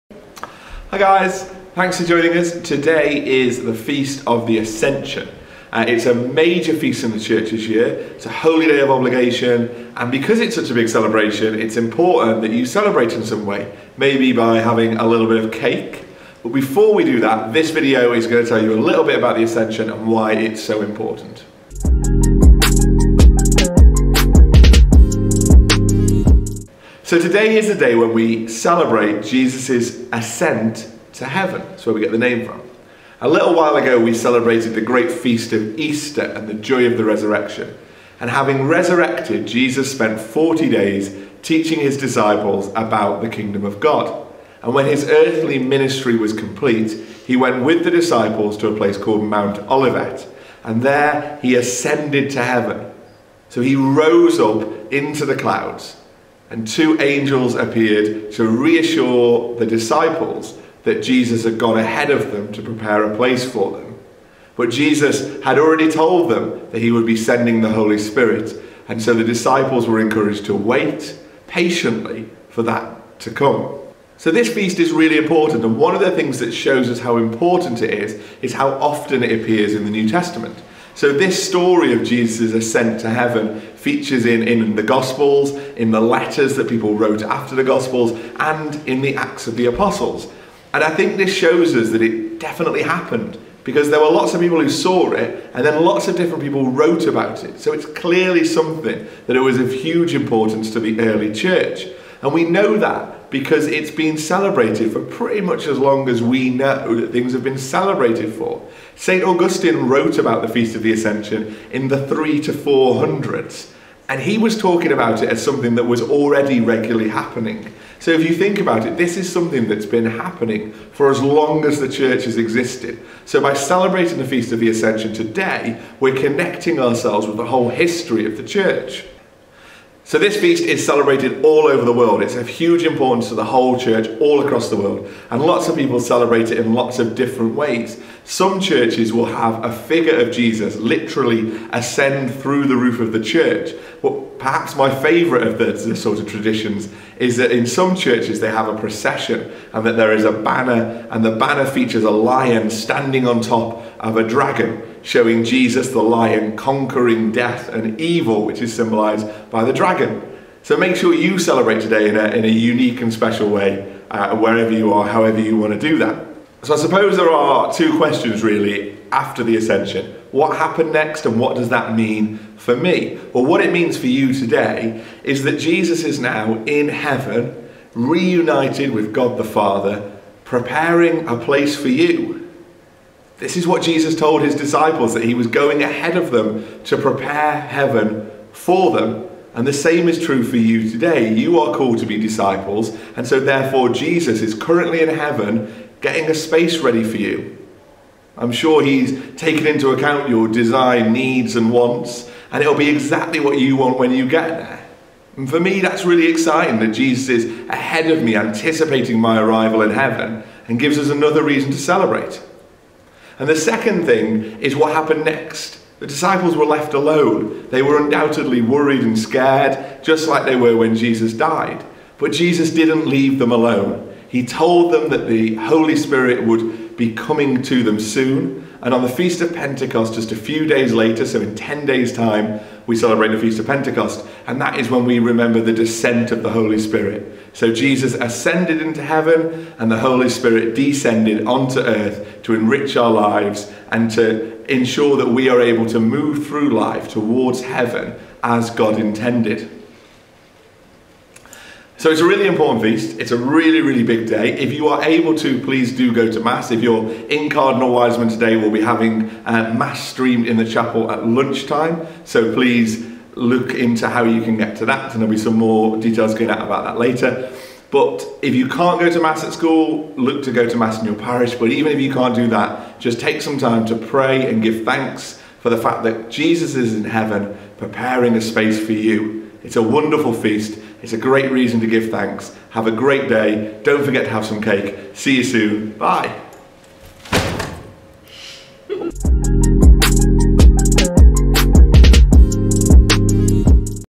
The Ascension of Jesus
Today is the Feast of the Ascension - the day we celebrate Jesus' return to Heaven. What's this day about and what does it mean for us?